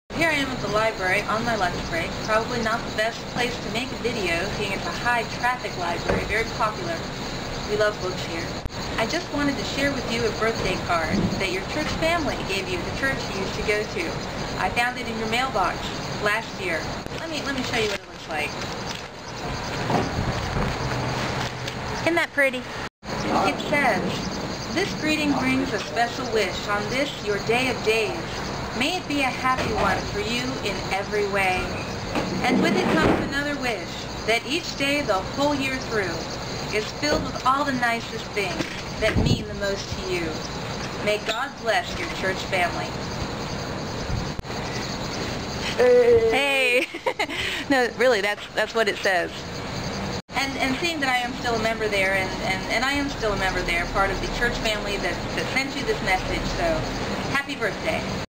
Better Late Than Never?
Although this card is last year's news, it's still this year's news. Happy Birthday!

If I make it back to that church, I'll check your box for this year's card. Maybe I can get it to you by Christmas this year :D